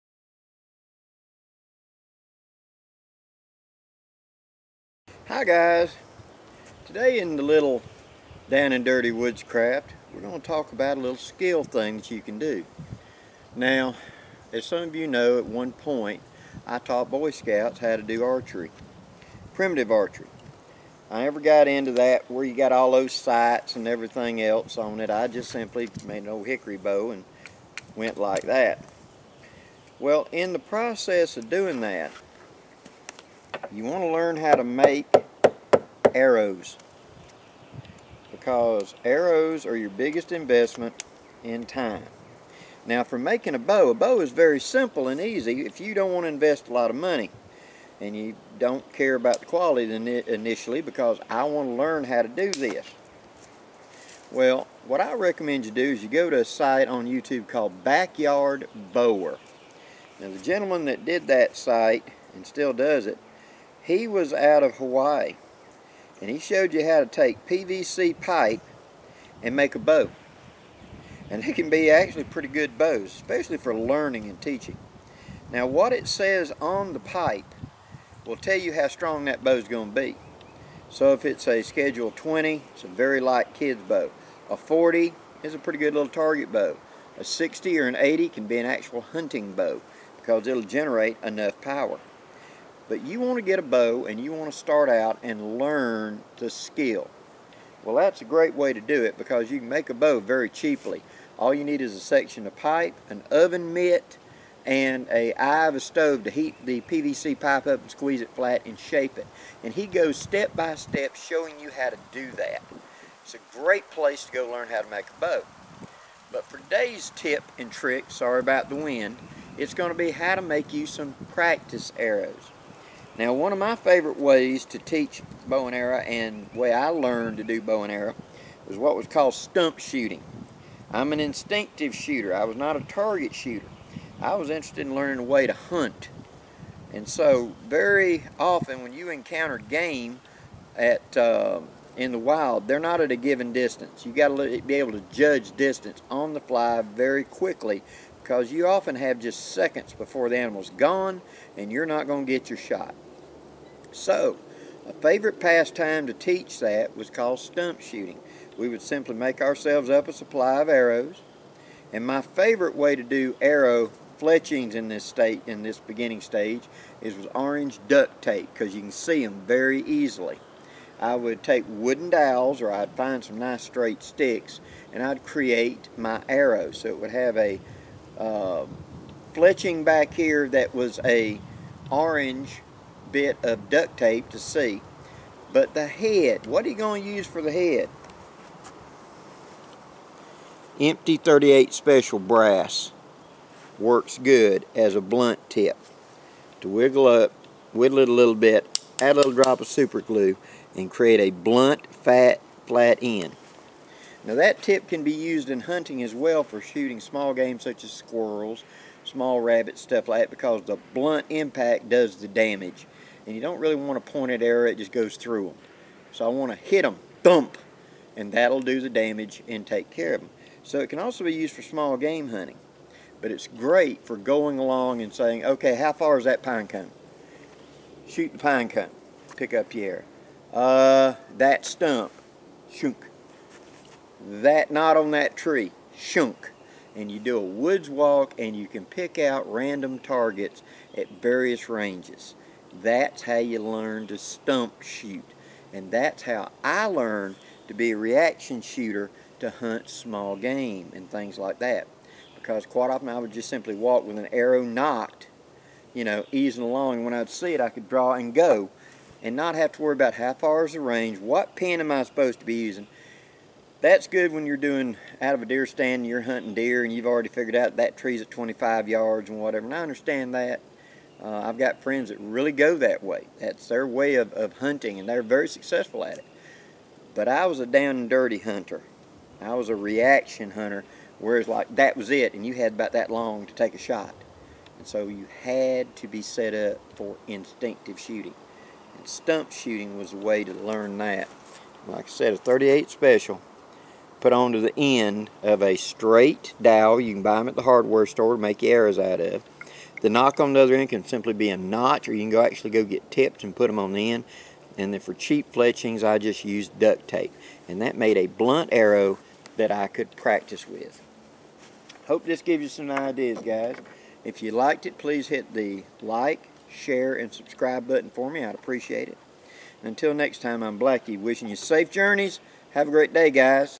DOWN N DIRTY CHEAP PRACTICE
IN SURVIVAL YOU NEED TO HAVE AS AMANY SKILLS AS YOU CAN TO COPE WITH THE TRIALS OF MAKING IT OUT ALIVE. ONE OF THE BEST SKILLS TO LEARN IS HOW TO SHOOT A BOW AND ARROW WELL. TO SAVE COSTS MAKING YOUR OWN ARROWS IS A GOOD IDEA. IN THIS VIDEO I POINY OUT ONE OF THE HARDER TO FIND PARTS OF A HOMEMADE ARROW..THE POINT. OR I SHOULD SAY IN THIS CASE A BLUNT TIP USED FOR TARGET WORK AS WELL AS SMALL GAME HUNTING.